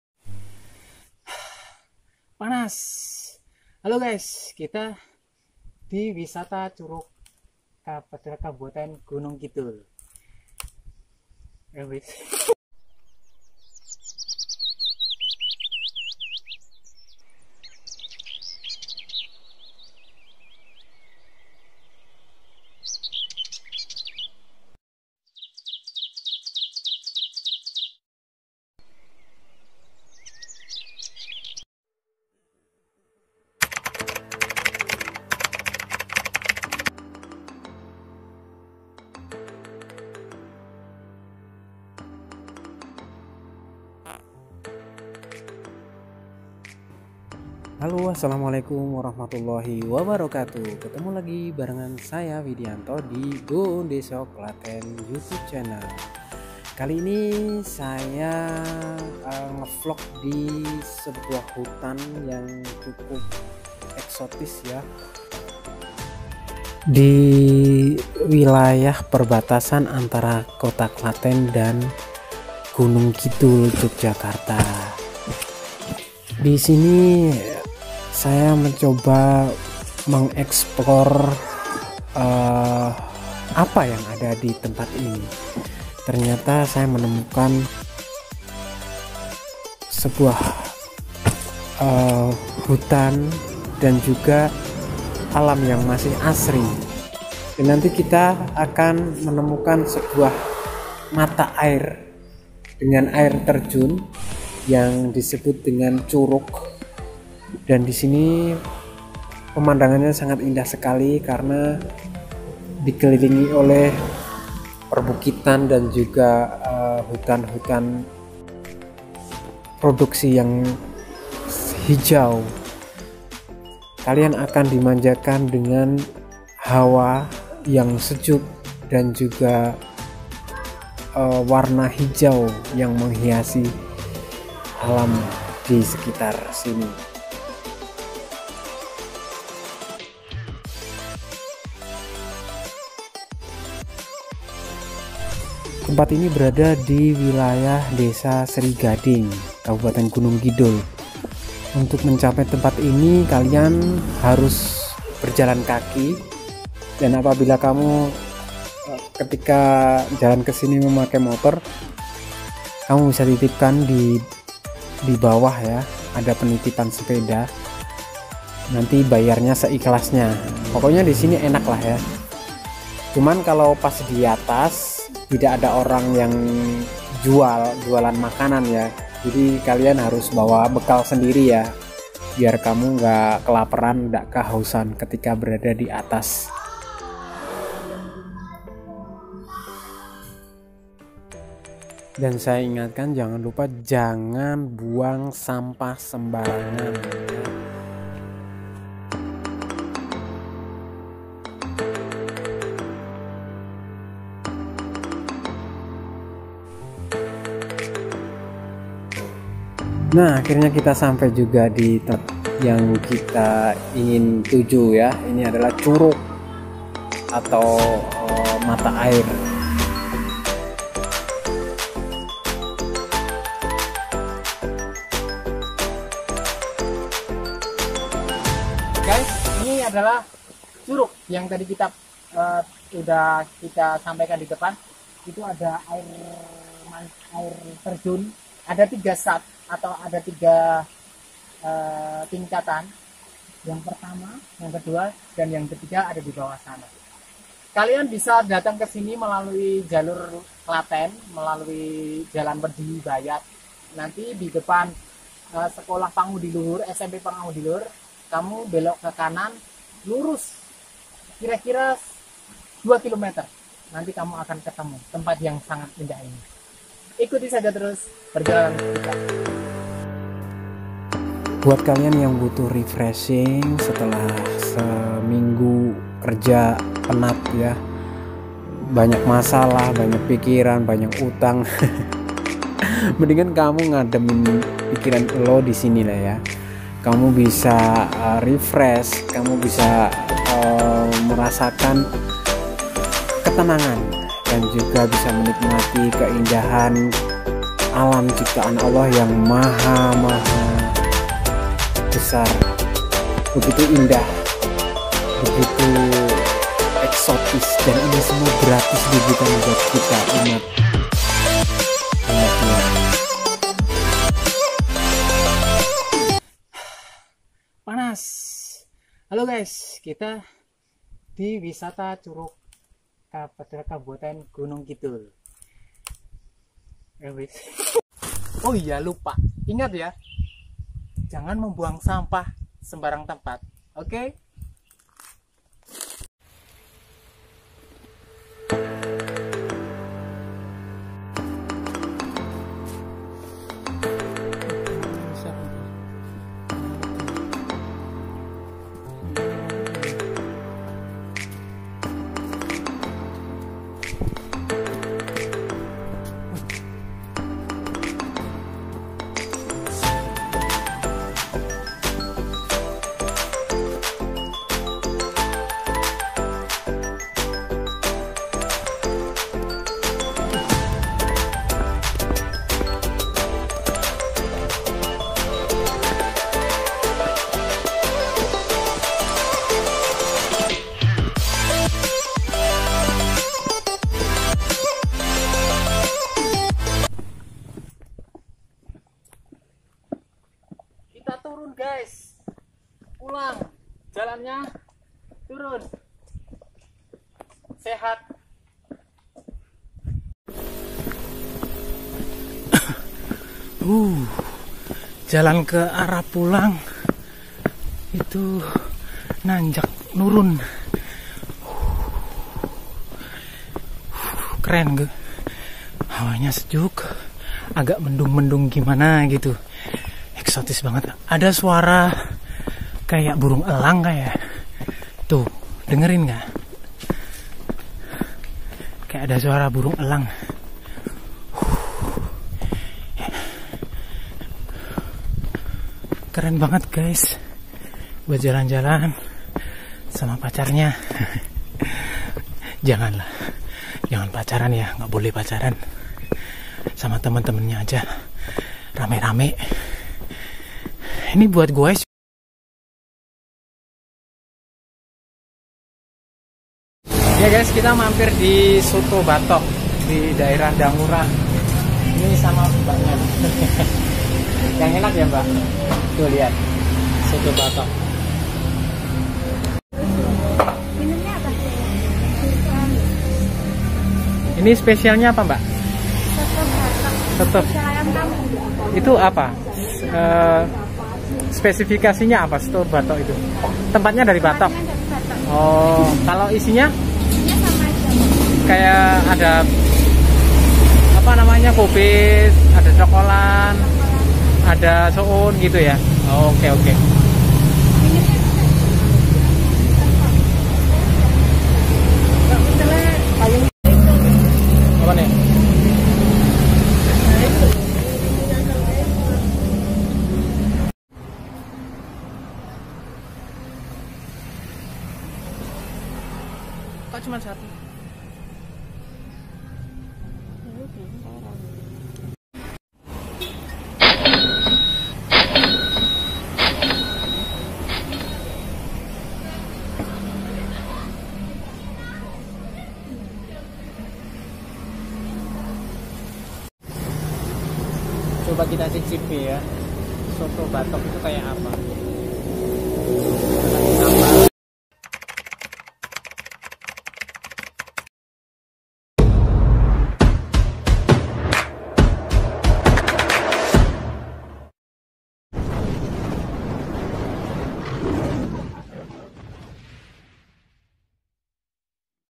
Air Terjun tersembunyi dibelantara Gunung kidul
sebuah Curug atau air Terjun yg mengalir dengan bebatuan besar yang sangat indah yang terdapat di desa Srigading kabupaten gunung kidul ini sangat recommended banget untuk dikunjungi selain indah dan masih sangat alami tempat ini tidak terlalu sulit untuk dijangkau hanya 30 menit dari kota Klaten kearah selatan.
tempat ini banyak sekali area untuk Selfi dan sebagai tempat untuk refreshing.
bersama @go-ndesoklaten7411  kalian akan kami ajak untuk mengexplore lebih dalam lagi apa yang ada ditempat ini. tonton terus GO-NDESO KLATEN youtube channel